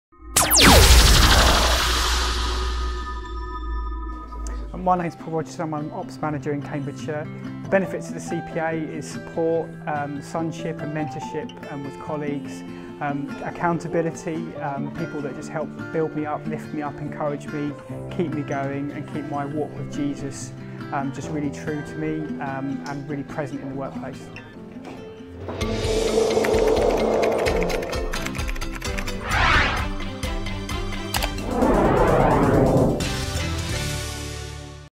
CPA 7 Paul Rogerson
A series of mini interviews asking members "What does CPA mean to you?"